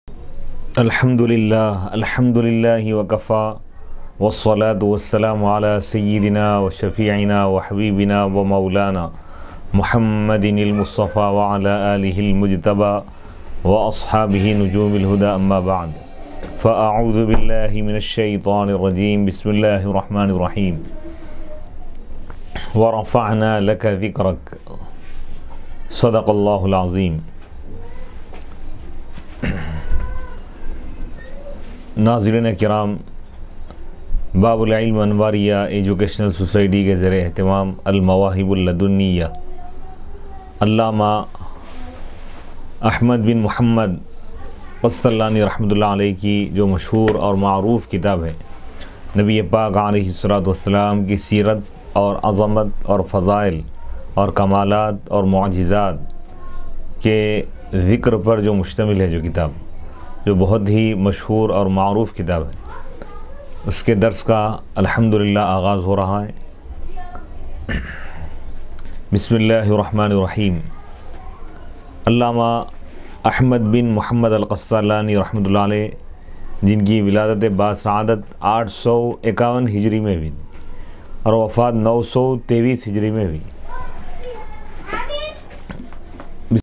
Allama Qastallani Ki Viladat Aur Wafaat | Al Mawahibul Ladunniyyah | ✍️ Allama Qastallani R.H.
Jamia Anwaria Hyderabad Presents Short Clip Of Mawahibul Ladunniyyah Lecture Series -

Please Find Below Full Link Of MUWAHIBUL LADUNNIYYAH Lecture 1

✍️ Allama Imam Qastallani R.H.

🗣️ Mufti Mohammed AmeenUddin Gold Medalist Maulvi Kamil Jamia Nizamia

Organised by Babul ilm Anwaria Educational Society